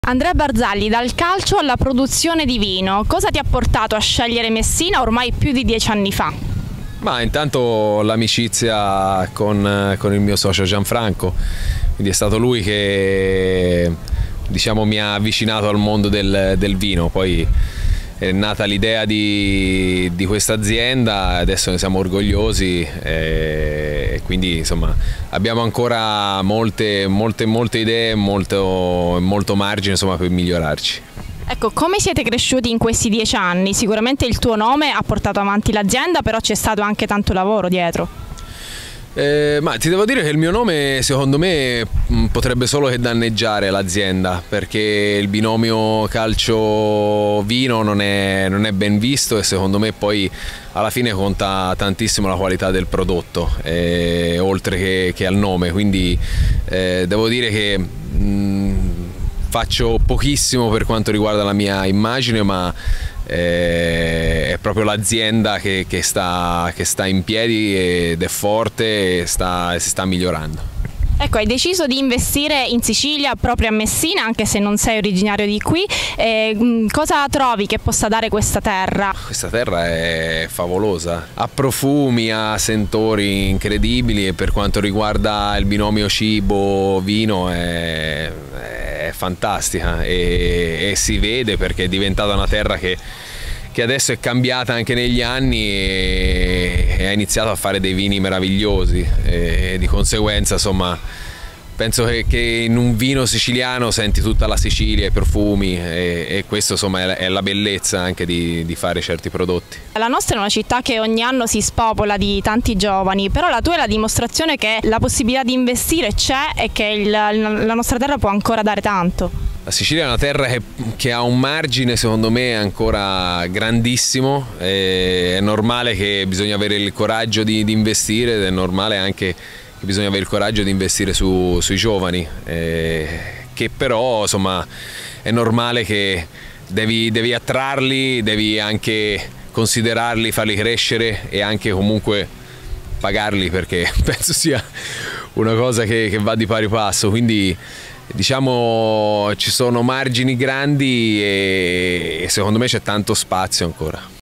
Andrea Barzagli e i vini messinesi: "Ho investito su questa terra 10 anni fa. Ne sono orgoglioso"
L'ex calciatore e campione del mondo ha fondato nel 2008, con un socio messinese, un'azienda vinicola a Faro Superiore. "La Sicilia è favolosa, bisogna avere il coraggio di investire"